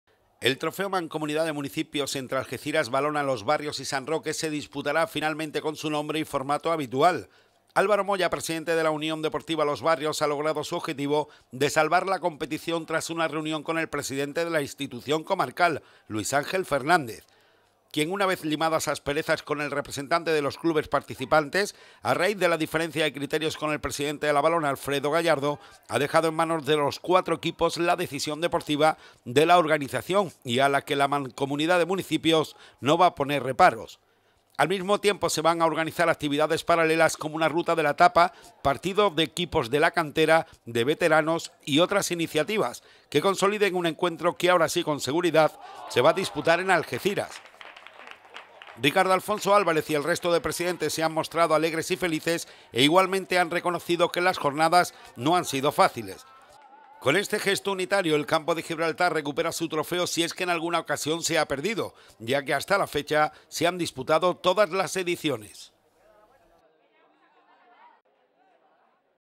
Algeciras acogerá la cuarta edición del Trofeo Mancomunidad.
Y en deportes, vuelven las aguas a su cauce y Algeciras albergará una nueva edición del Trofeo campo de Gibraltar. Una competición veraniega que no alterará su formato inicial y que tendrá a los locales, la Balona, el San Roque y Los Barrios como participantes.